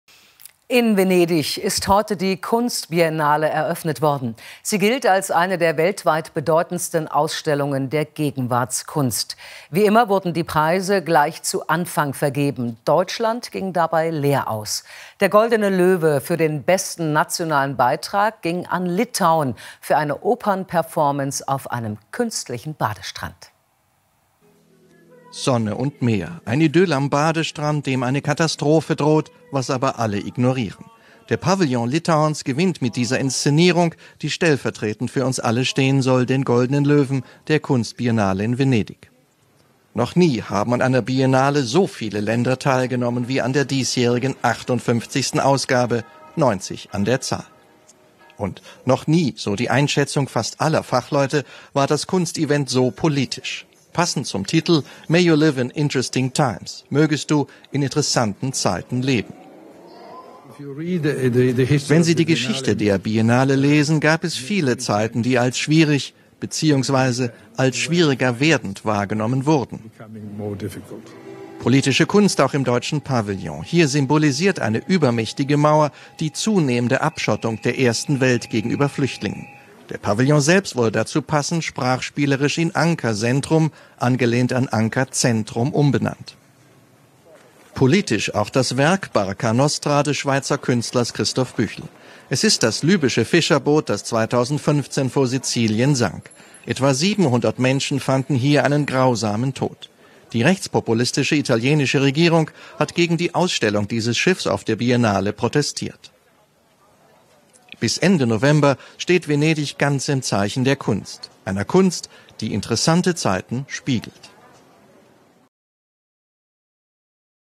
58. Biennale für Publikum - Kunstausstellung in Venedig, eröffnet 11.05.2019
Kunstausstellung in Venedig: Biennale öffnet für Publikum